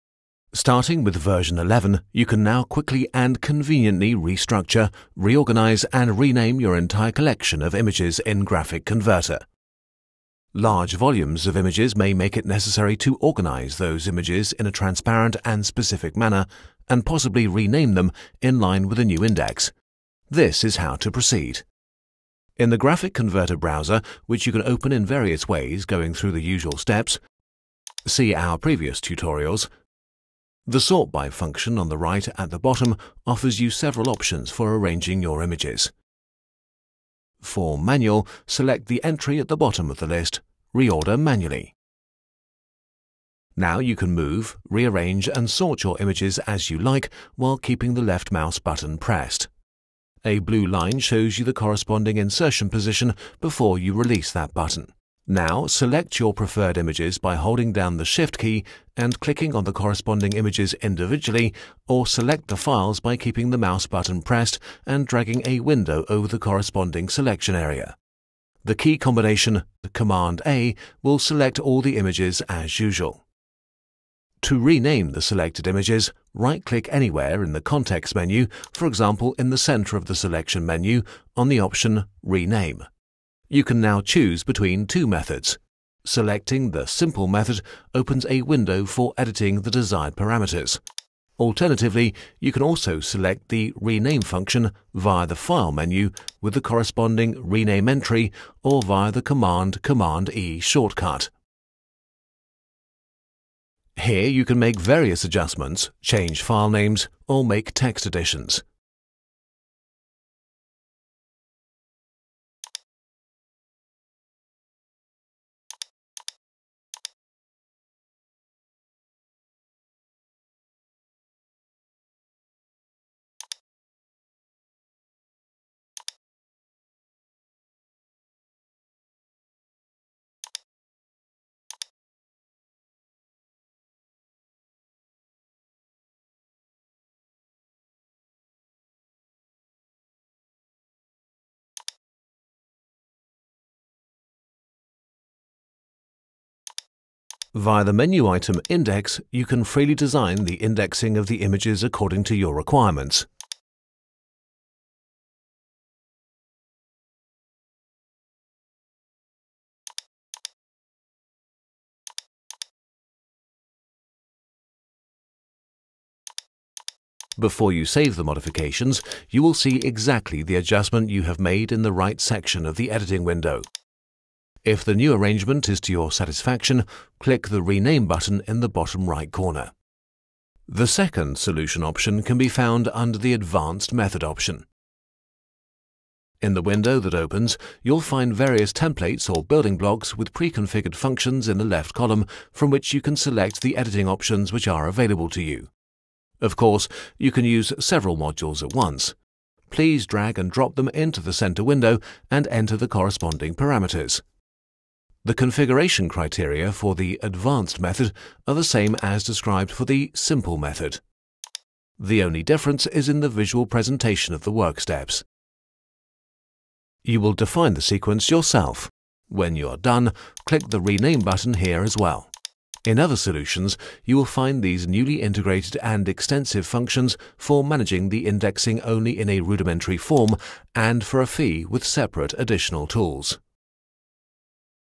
Manual sorting and renaming of images in GraphicConverter 11
Starting with version 11, you can now quickly and conveniently restructure, reorganize, and rename your entire collection of images in GraphicConverter.
Large volumes of images may make it necessary to organize those images in a transparent and specific manner, and possibly rename them in line with a new index.
Now you can move, rearrange, and sort your images as you like. while keeping the left mouse button pressed. A blue line shows you the corresponding insertion position before you release that button.
Here you can make various adjustments, change filenames or make text additions. You can freely design the indexing of the images according to your requirements.
With a new advanced and object-orientated method, it is possible to arrange various templates or building blocks with preconfigured functions, from which you can select the editing options which are available to you.